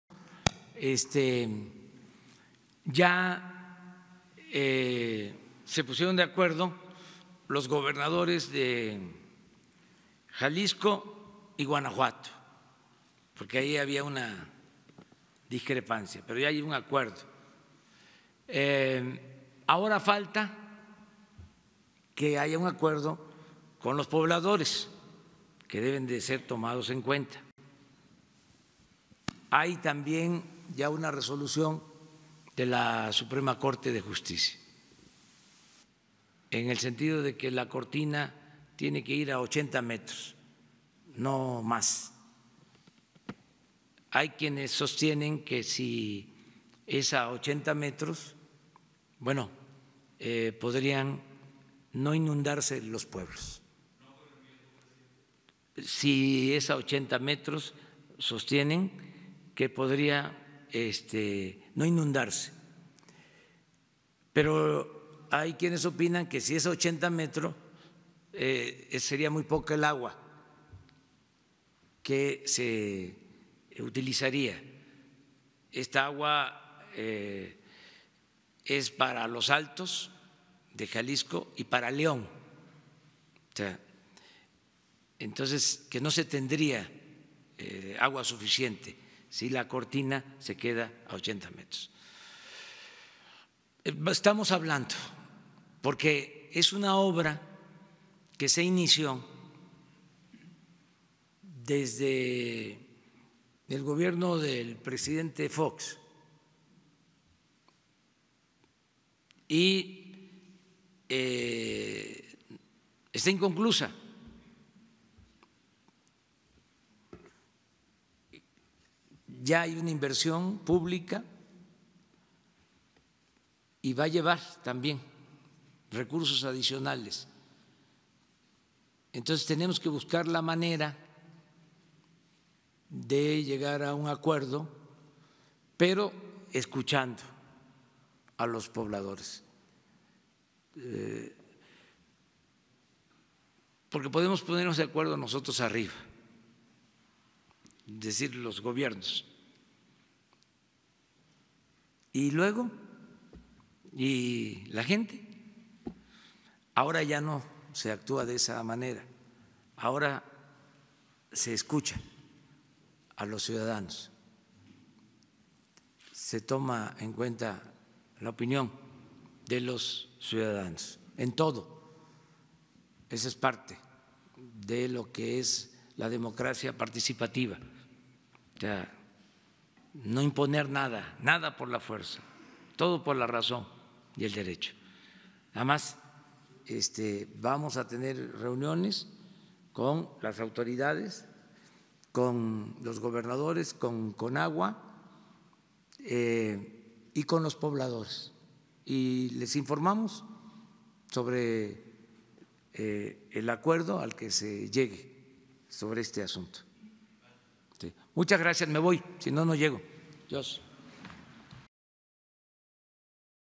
Presa El Zapotillo ¿inviable? Responde AMLO - Sandy Aguilera
No hay una decisión tomada, se ha solicitado que se revise acorde a derechos humanos, ambiental y jurídico el proyecto de presa el Zapotillo